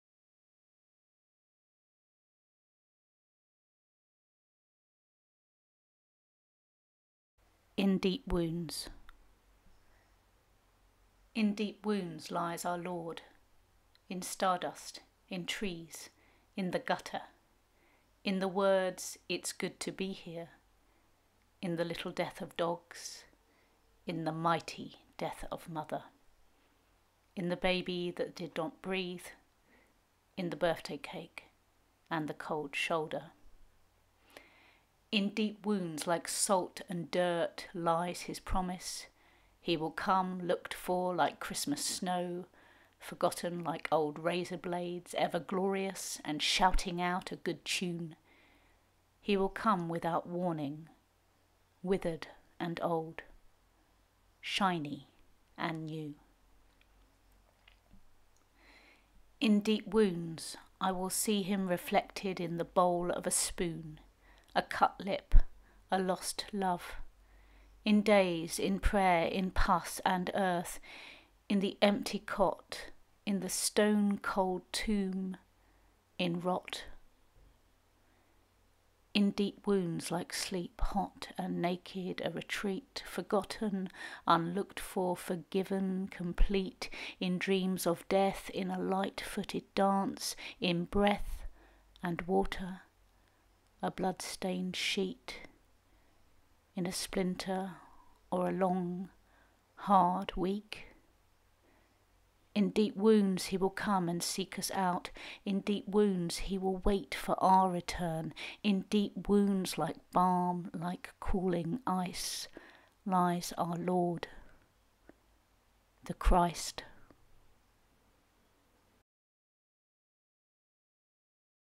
Good Friday - Holy Week Reflections
In Deep Wounds

In deep wounds lies our Lord.
In stardust, in trees, in the gutter.
In the words, ‘it is good to be here.’
In the little death of dogs, in the mighty death of Mother.
In the baby that did not breath,
in the birthday cake and the cold shoulder.

In deep wounds like salt and dirt lies his promise.
He will come, looked for like Christmas snow
… forgotten like old razor blades.
Ever glorious and shouting out a good tune.
He will come without warning withered and old … shiny and new.

In deep wounds, I will see him reflected in the bowl of a spoon,
a cut lip, a lost love.
In days, in prayer, in pus and earth,
in the empty cot, in the stone-cold tomb.
In rot.

In deep wounds like sleep, hot and naked,
a retreat. Forgotten,
unlooked for, forgiven, complete.
In dreams of death, in a light-footed dance, in breath and water.
A blood-stained sheet.
In a splinter or a long hard week.
In deep wounds he will come and seek us out.
In deep wounds he will wait for our return
In deep wounds like balm, like cooling ice
lies our Lord … The Christ.

Throughout Holy Week 2020 we will release a poem by Em Kolltveit and image by Janice Issitt each evening, reflecting on this strange and different Holy Week that we are experiencing in 2020, remembering Jesus's journey to the cross and his resurrection. Janice is in isolation in rural Suffolk and Em is in London.